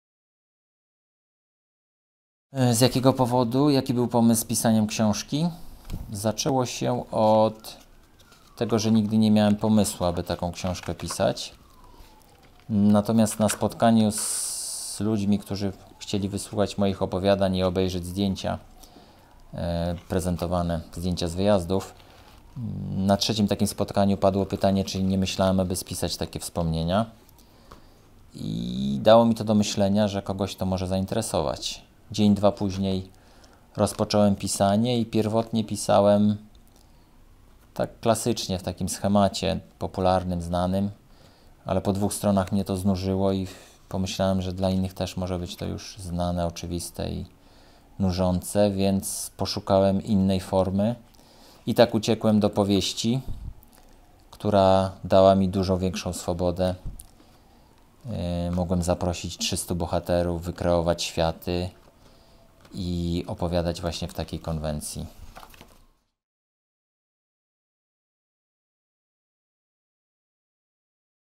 ODLUDEK. SoliTARY SOLDIER - Wywiad z autorem - Robert Kulik, odc.1
ODLUDEK. SoliTARY SOLDIER.
Wydawnictwo Port Arthur

Premiera: 20 października 2020 r.
Książka dostępna w przedsprzedaży.

Z cyklu „WYWIAD z AUTOREM”
Robert Kuliki, autor powieści ODLUDEK. SoliTARY SOLDIER, odsłania kulisy procesu twórczego i warsztatu pisarza, dzieli się przemyśleniami na temat losu debiutanta, opowiada o niuansach wyłaniania się fabuły i bohaterów książki.
Premiera ODLUDKA: 20 października 2020 r.
O czym jest „ODLUDEK”?